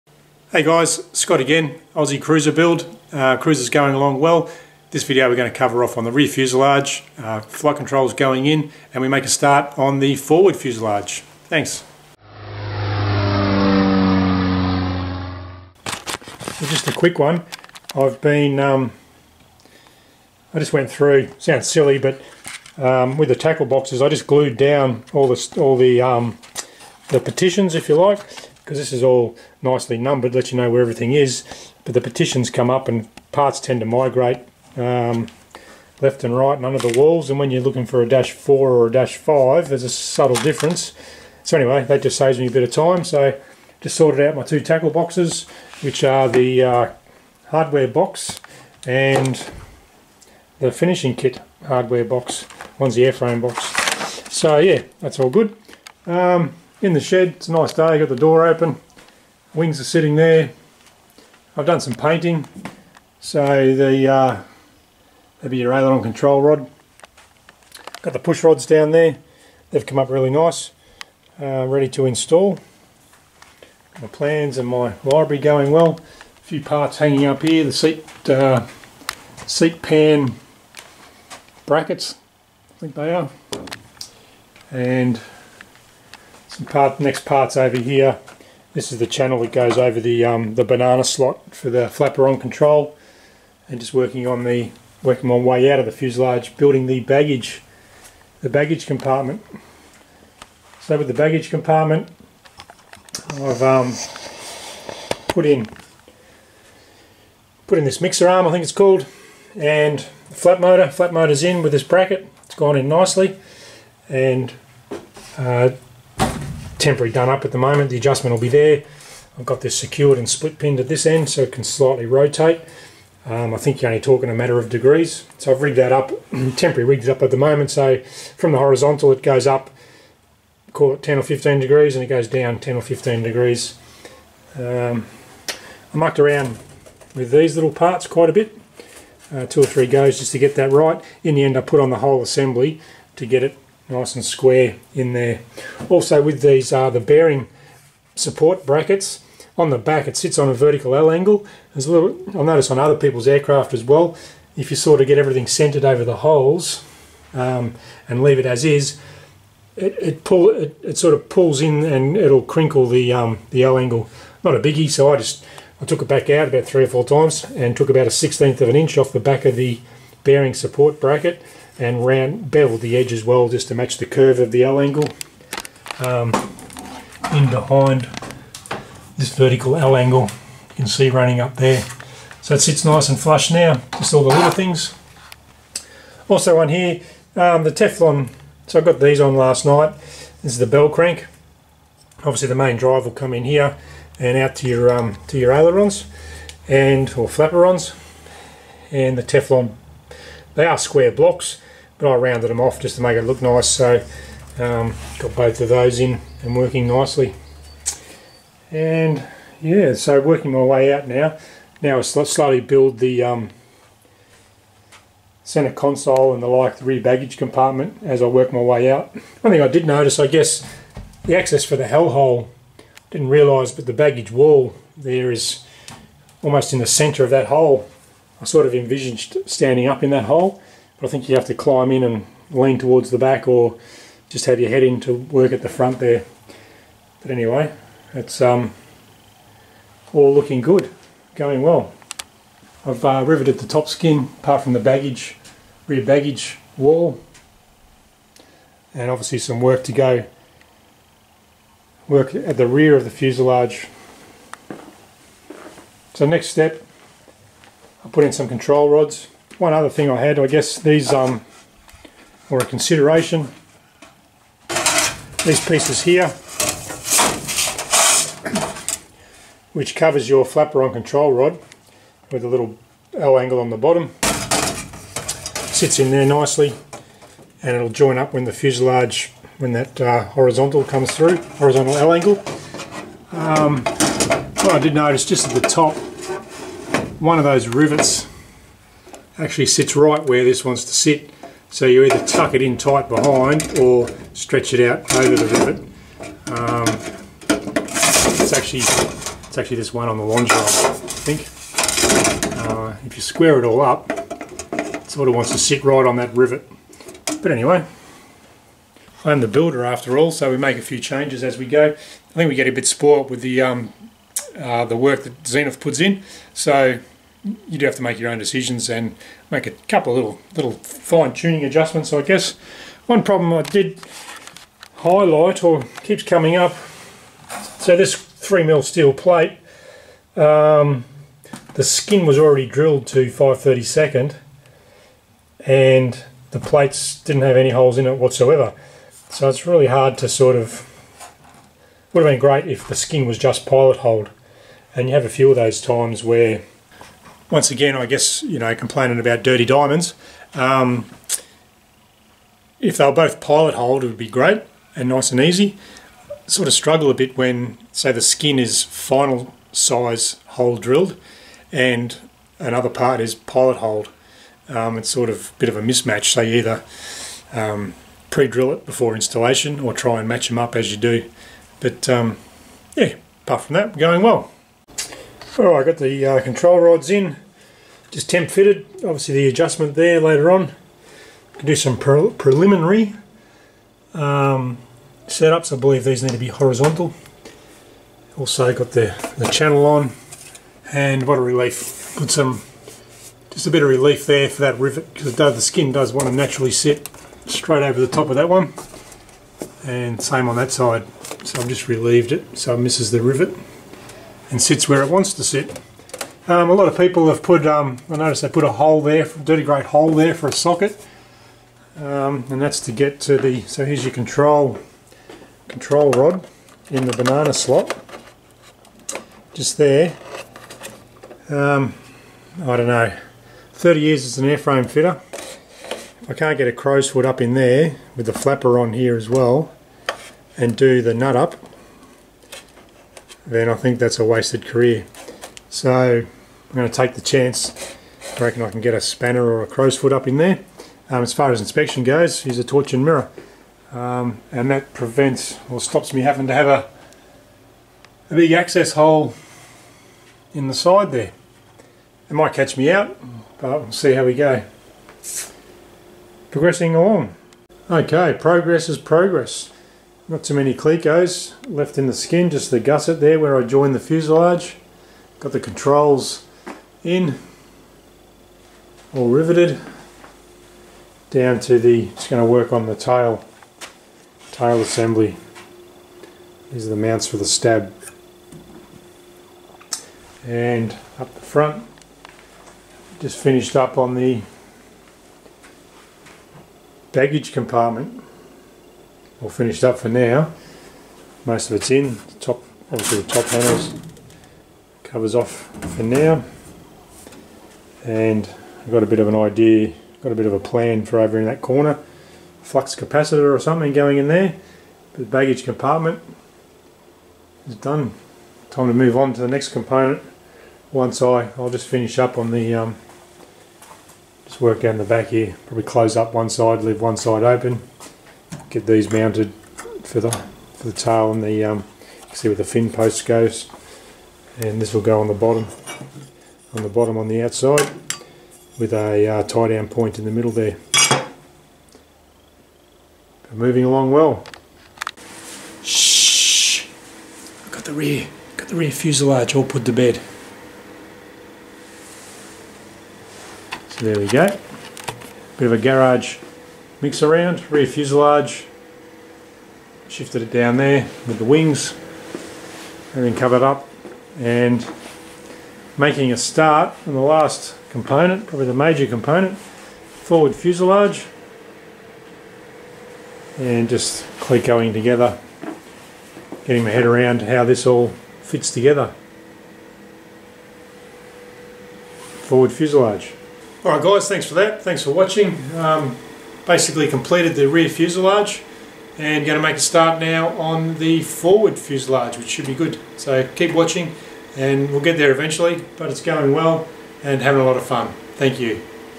How I assembled the Aussie 750 Zenith Cruzer Rear fuselage May 20
Construction of the rear fuselage Zenith 750 Cruzer.